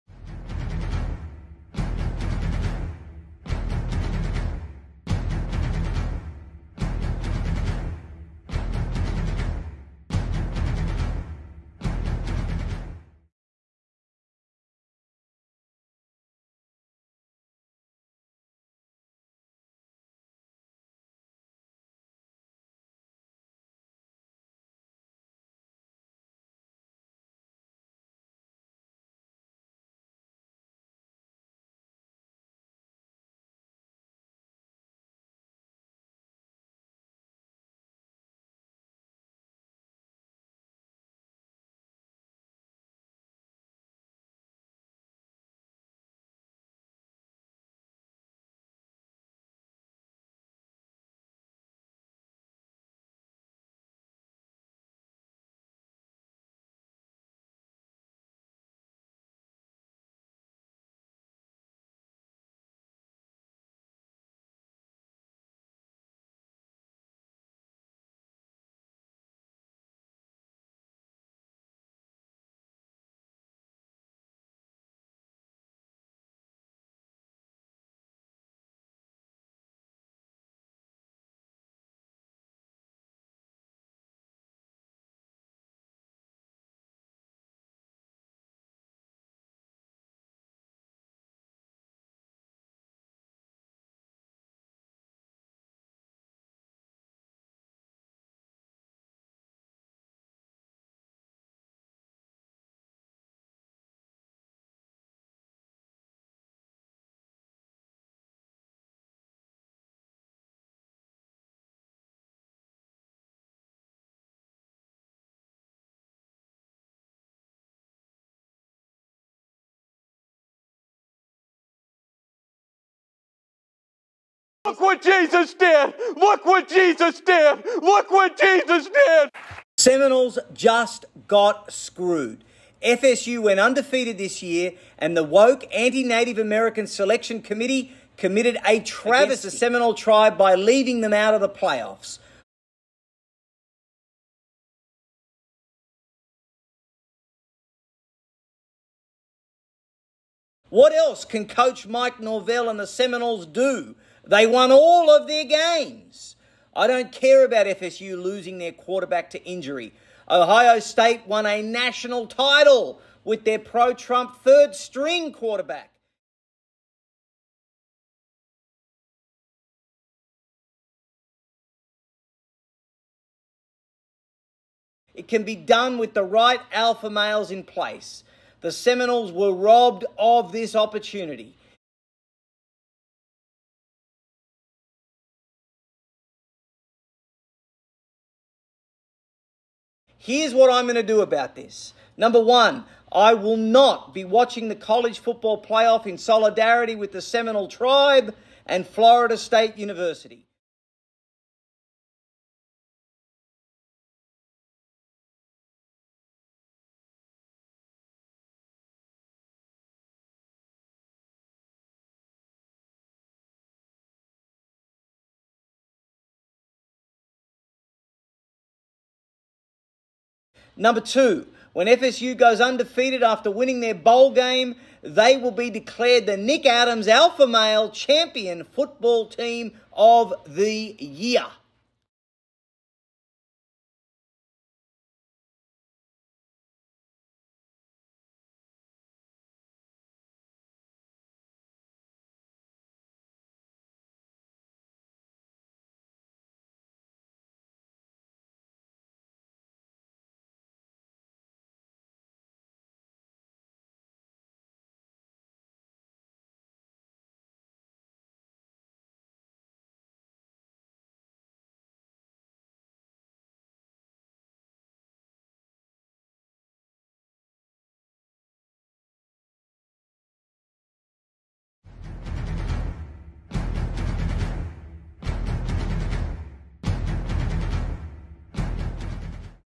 CTP Know The Truth Quickie Nikki Boycotts CFP
Self-appointed, alpha male Nick Adams is boycotting the college football playoff because the Florida State Seminoles did not make the final four.